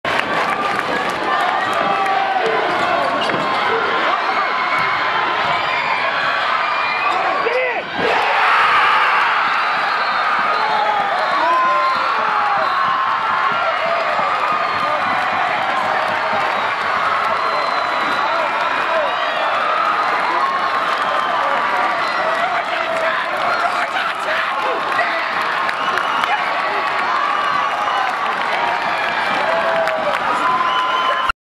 Shaker beats CBA, 12/3/05
Brad Sheehan hits the winning shot with 4 seconds remaining as the Bison defeat CBA, 46-45.